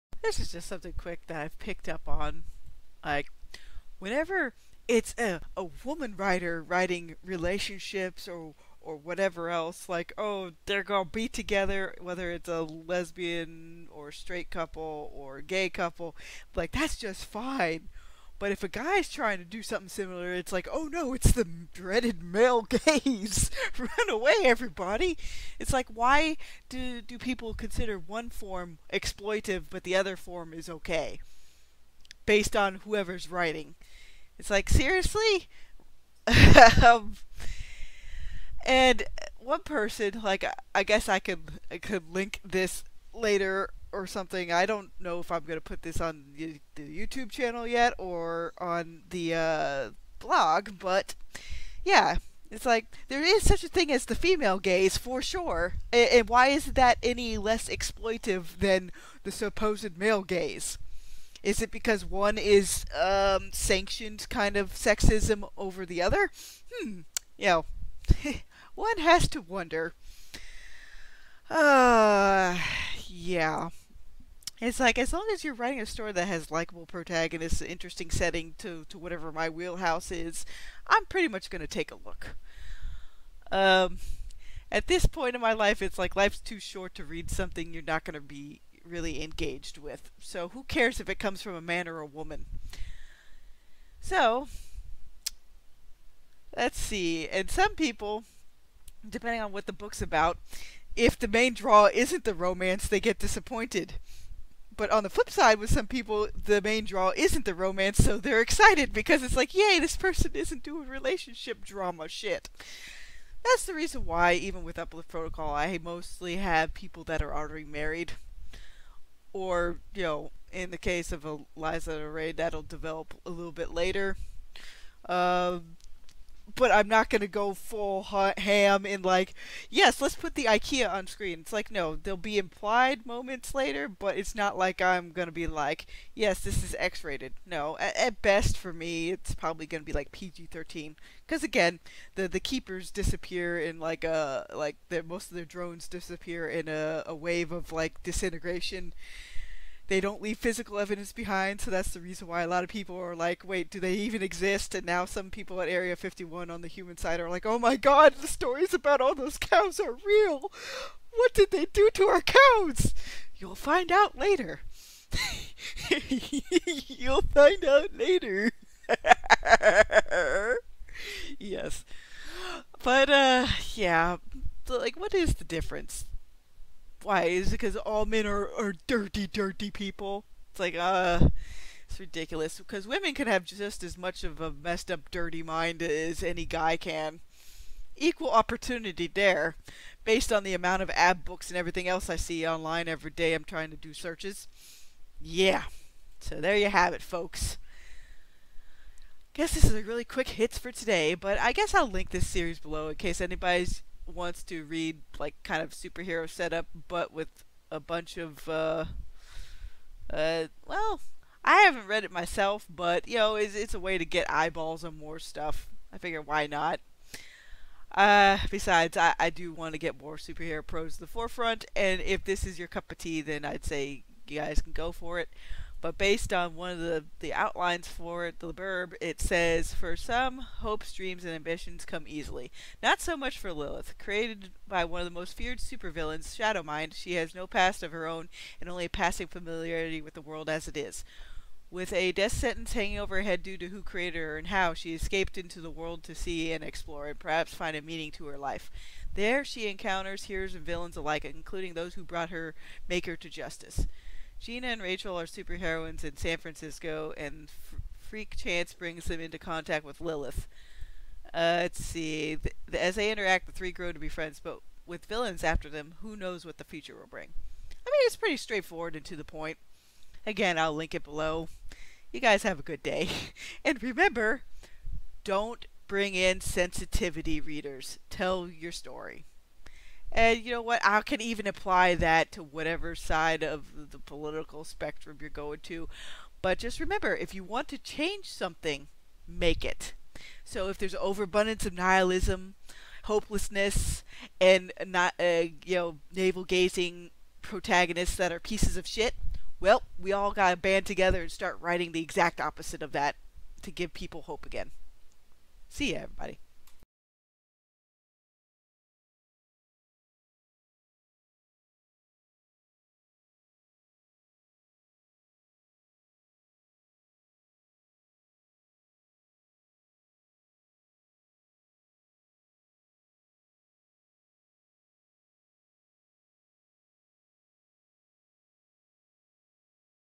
The Male Gaze vs The Female Gaze
What gives women writers the ok vs "Oh no! Don't write that you nasty male!"
This reader wants likeable protagonists. I dont care what who or what a writer is as long as I get that.

Edit: I did a followup here, if anyone is interested.